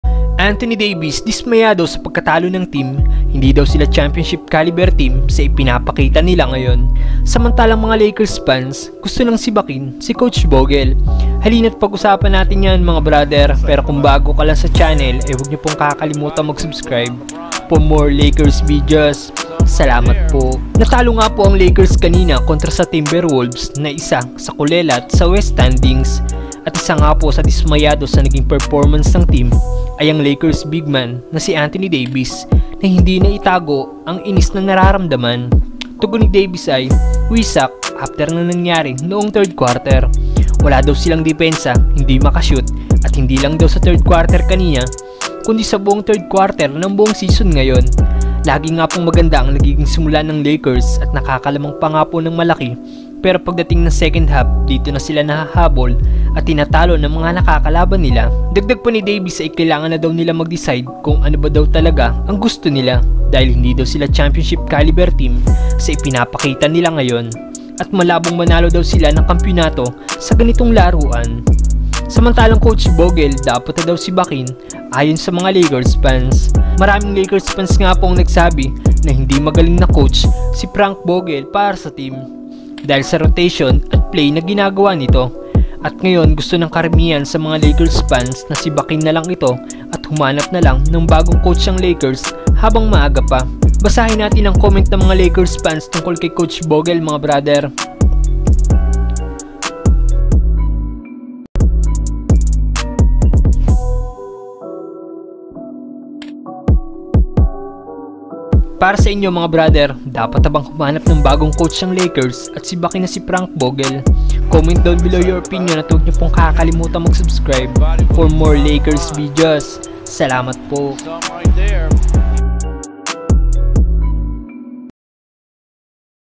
NAKU! DAVIS GALIT NA SA TEAM! | COACH VOGEL DAPAT TANGGALIN NA DAW AYON SA MGA LAKERS FANS!
PERPLE SPORTS TV